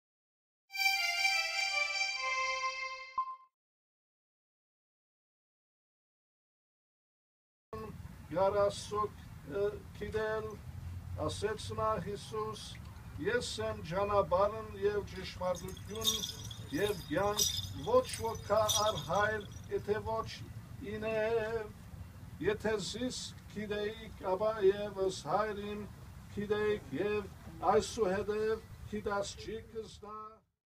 Requiem service in Fresno to remember Armenian genocide
Requiem service was held Thursday at Ararat Cemetery’s Memorial of the Unknown Armenian Martyr, one of the events marking the centennial of the start of the Armenian genocide.